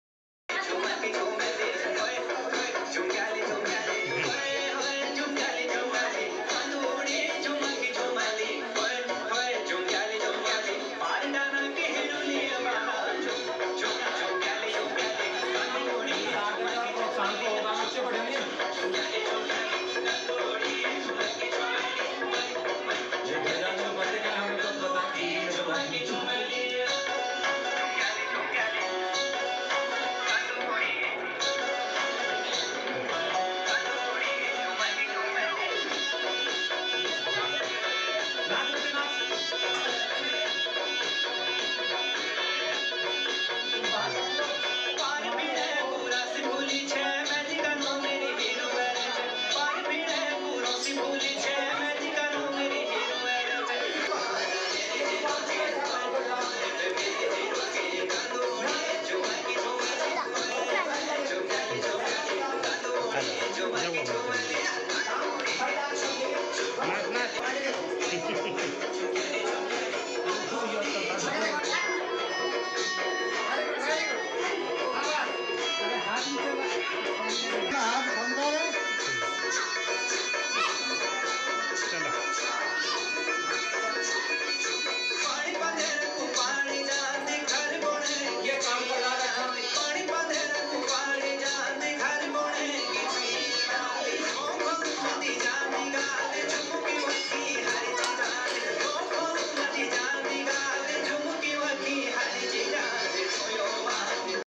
School k bacho ne kiya Garhwali gane pe kiya kamal ka dance/uttarakhand dance
Video from Dinesh MadhwalSchool k bacho ne kiya Garhwali gane pe kiya kamal ka dance/uttarakhand dance